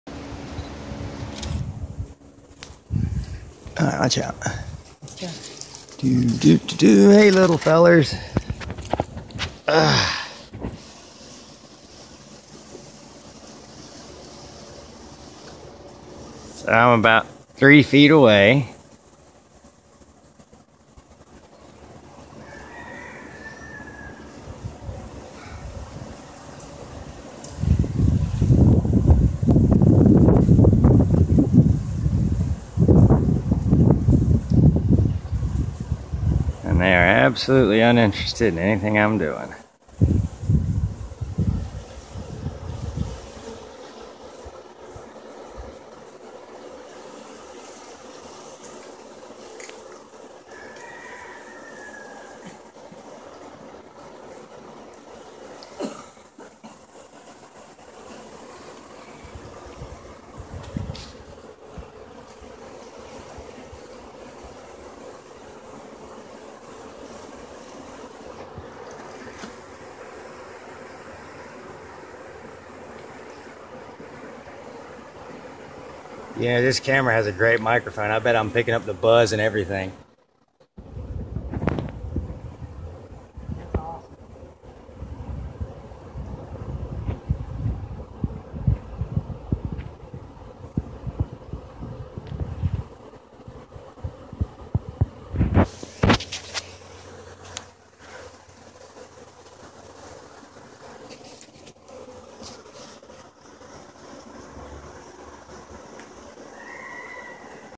Our Buckfast Sweetheart Bees
Me, with no protection or smoke, within 1 m of our super docile Buckfast Bees while they are very active and busy.  Hope this shows people the truth about bees and their attitude towards humans.